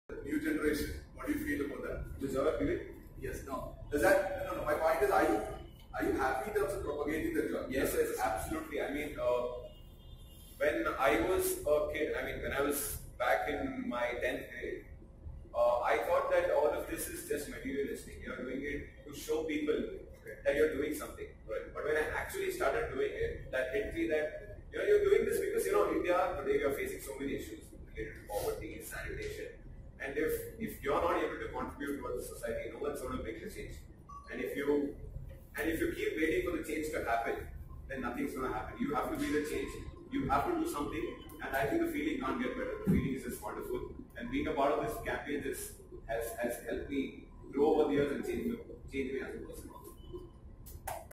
Gaurav Joshi from Jain University interaction with Shyam Menon, Fourth Ambit
An exchange between Shyam Menon, Cofounder, Fourth Ambit, and Gaurav Joshi, student, Jain University on different generations and the prevalence of social entrepreneurship